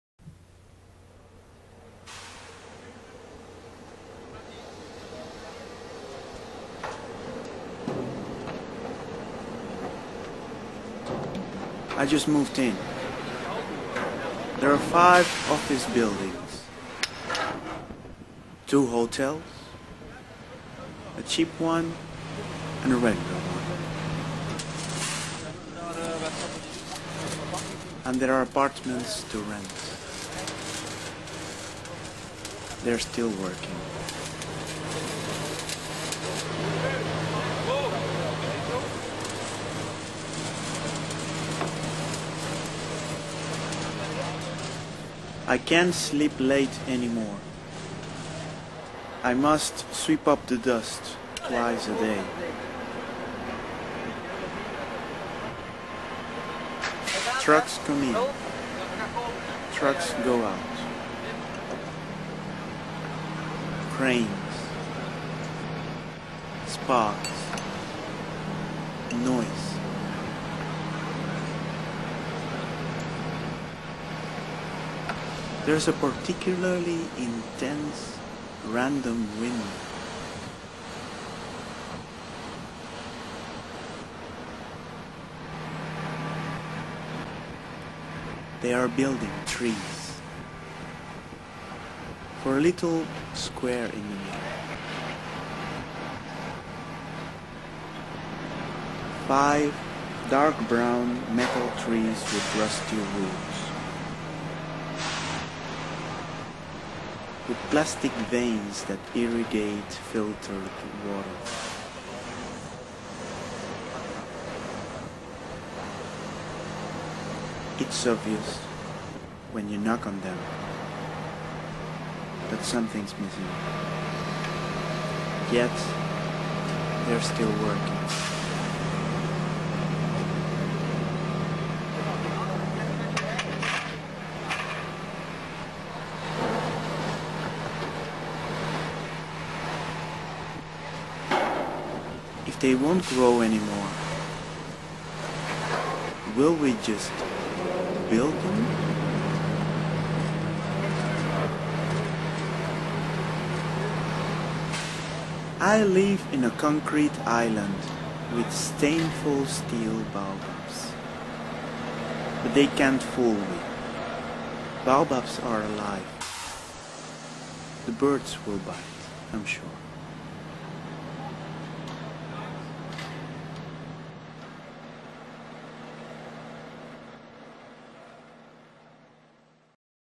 Stainful Steel
Stainful steel is based on images that have been recorded during the construction of an art work in the middle of a recently developed office center in Antwerp, Belgium. Next to the Central Train Station, the Kievitplein, a new business neighbourhood has been created with four buildings for a major multinational, one building for regional government offices, one budget hotel, one luxe hotel and one building with appartments for rent. Given the protest of the neighbours, it was promised to be also a meeting point for the people, but it has not a single green space.

There is a square planned for the middle of that space. And what is most ironical is that the space has been assigned for an art work in the form of five baobabs of rusty steel.

The irony comes from the fact that baobabs are an ecological statement themselves. In the arid West Africa, they are able to store hundreds of liters of water. Babobabs can survive the most dramatic living conditions and as opposed to most trees, their wood is not solid, but soft instead. Almost like flesh.

However beautiful, the steel trees are themselves a sign of how ridiculous human pretentiousness can be when we face the possible extinction of life: the trees are empty.

Have we accepted that trees will die and are we substituting them?

Or have we accepted that trees are dead and are we building statues for them?